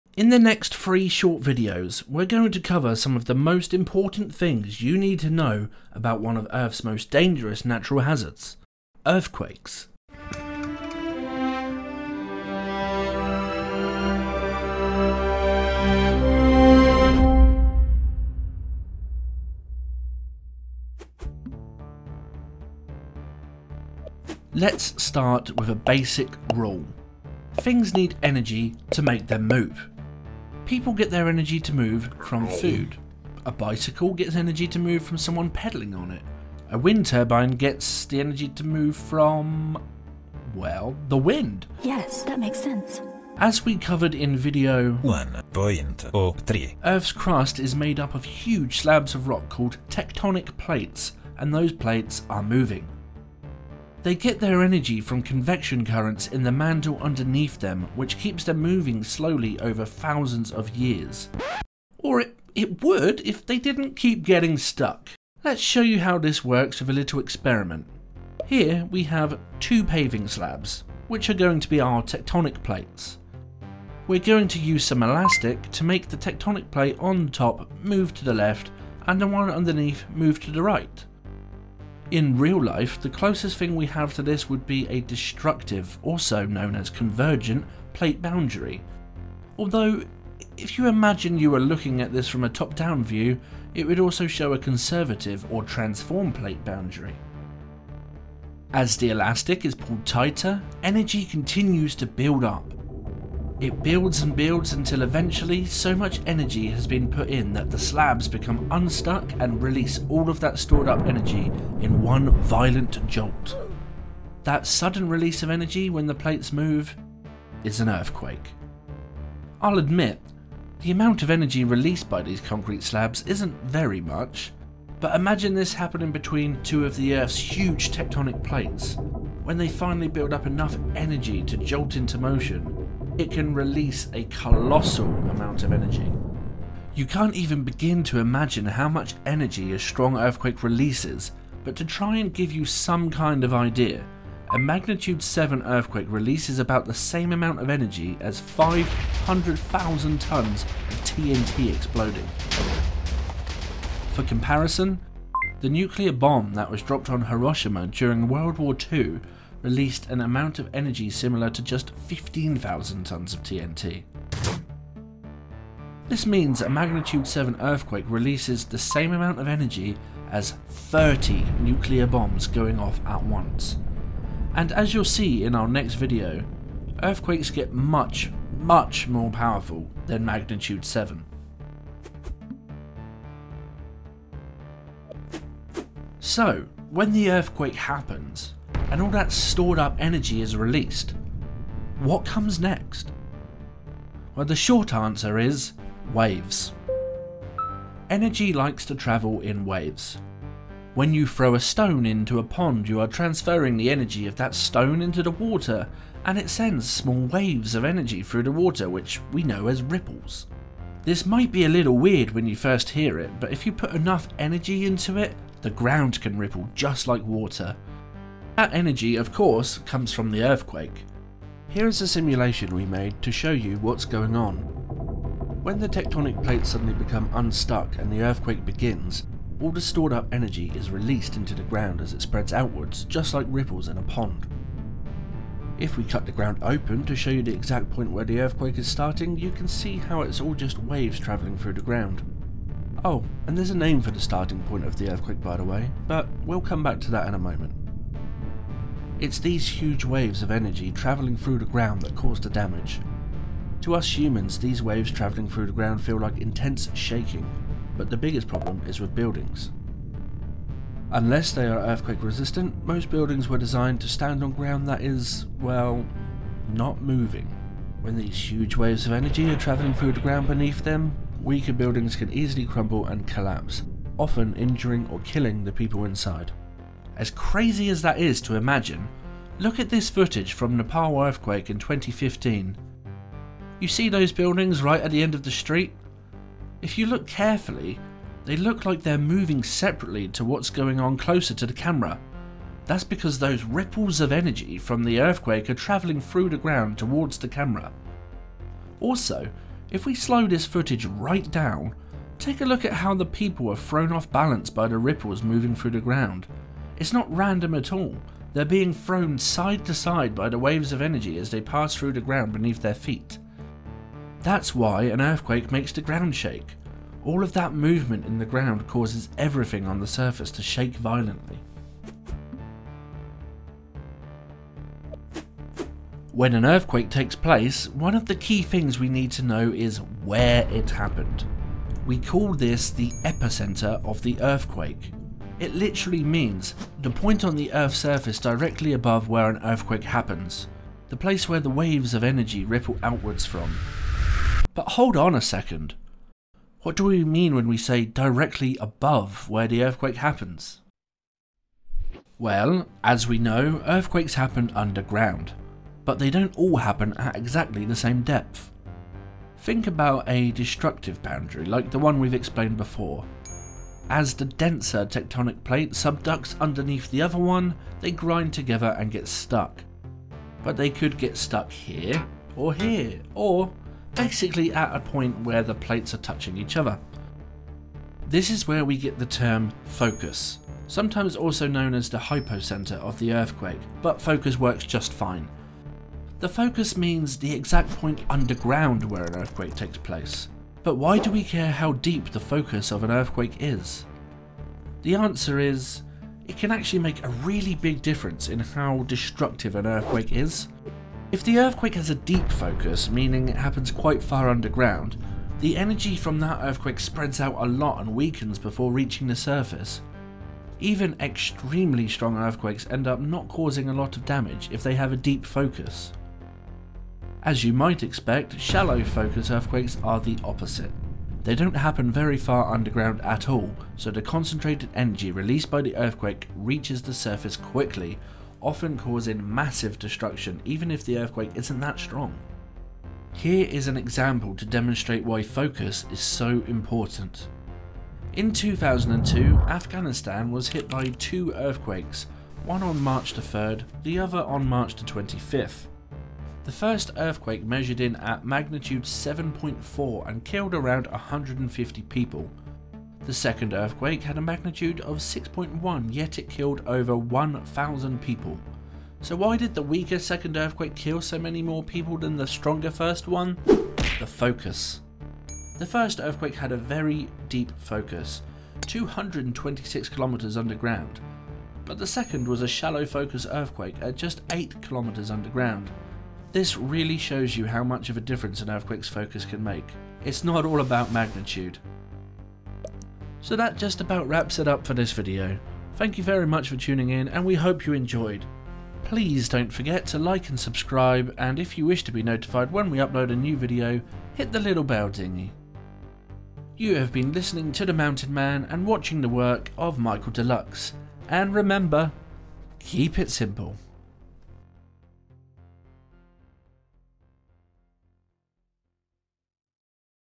Earthquakes: What, How and Why?
In this video we discuss Earthquakes some more.
What are they?
How do they happen?
Why are they all so different?

Join us in the wobbly world of Earthquakes!

00:00 Introduction
00:18 Earthquakes explained: Part 1
03:04 Why does the ground shake?
05:24 The Epicentre and Focus
08:03 Outro